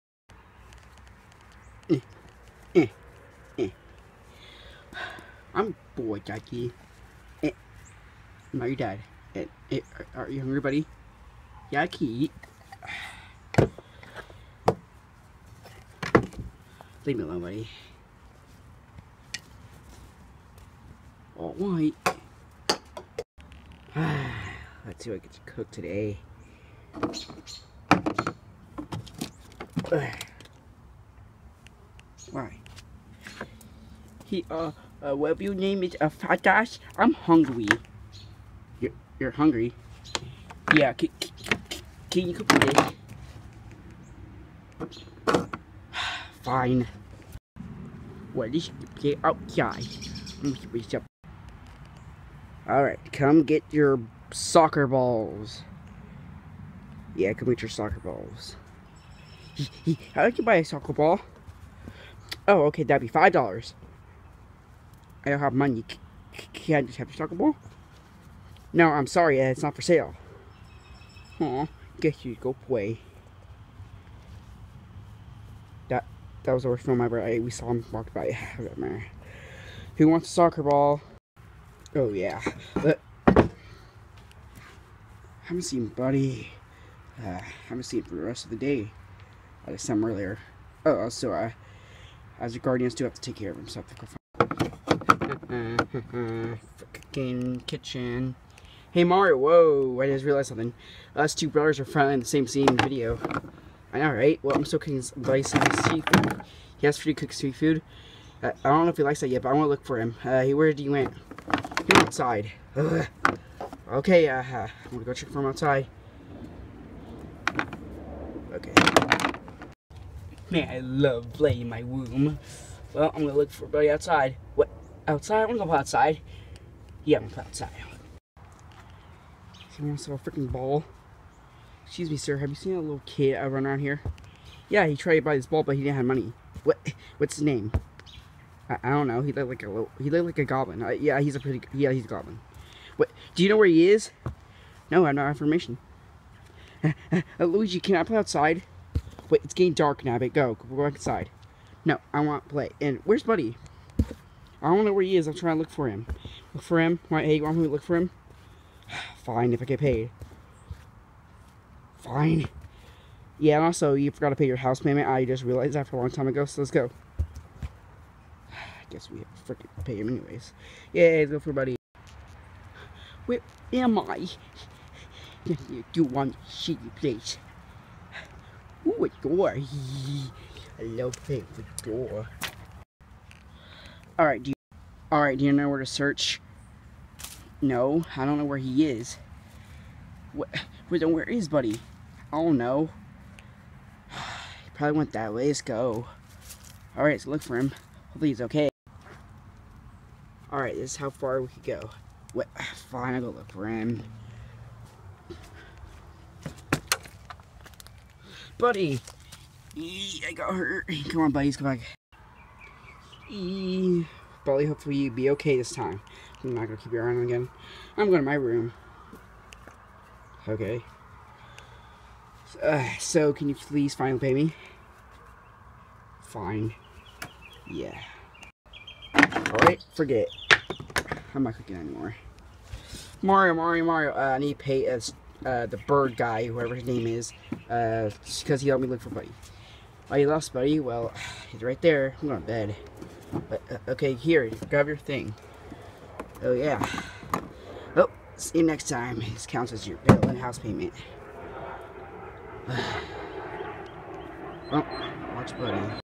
CG2 movie: Buddy is Missing
Buddy goes out side and gets lost but luigi,nabit,and bird dude guy? Go find him